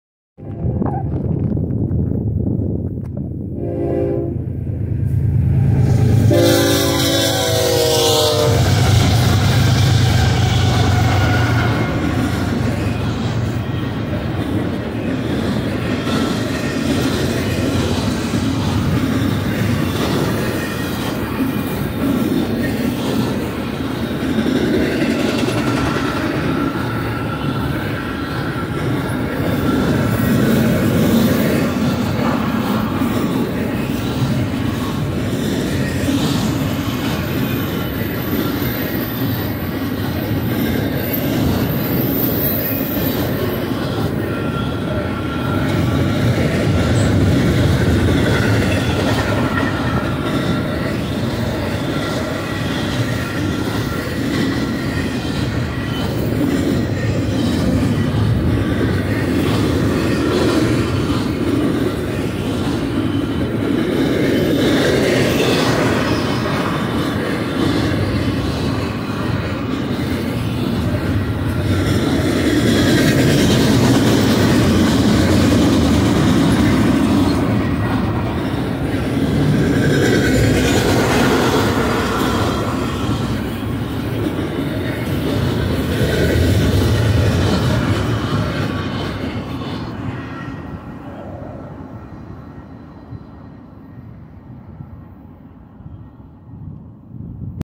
Fast EB BNSF Domestic Intermodal At Nebo Ca!
This eastbound BNSF domestic interomodal train highball here at Nebo Ca!

Here you will see in this train video is BNSF domestic intermodal train like JB Hunt Intermodal, C.H. Robinson, Amazon Prime, FedEx Multimodal, Martin,  ESTES, KLLM Transport, COFC, Walmart. Com, CSX Intermodal How Tomorrow Moves, Prime Intermodal, Double Stack Logistics, R+L Carriers, ABF Freight, Axsun, Highlander Intermodal and Unit 53.com

6552-ES44C4 (BNSF) (Leading Unit)
5904-ES44AC (BNSF) (Trailing)
6774-ES44C4 (BNSF) (Trailing)
6133-ES44C4 (BNSF) (Trailing)